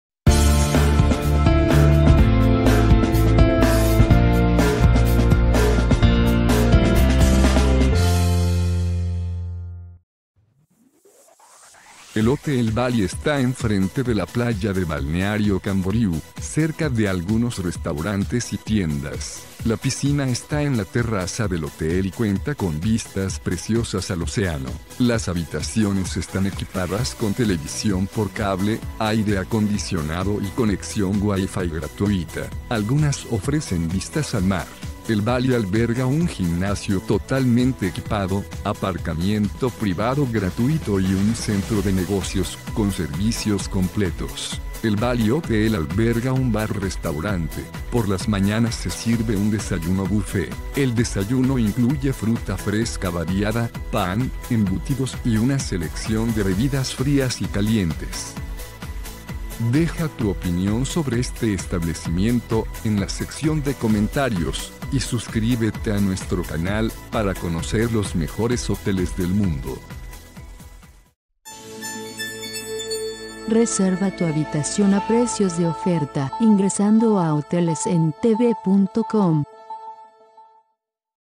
Hotel Bhally en Balneário Camboriú
Hotel Bhally cuenta con restaurante, salas de reuniones / banquetes y bar. El edificio se ubica en Av. Atlântica, 3250 y cuenta con habitaciones con minibar, ducha y caja fuerte. Hotel Bhally es una excelente alternativa para visitantes que quieran recorrer la ciudad de Balneário Camboriú.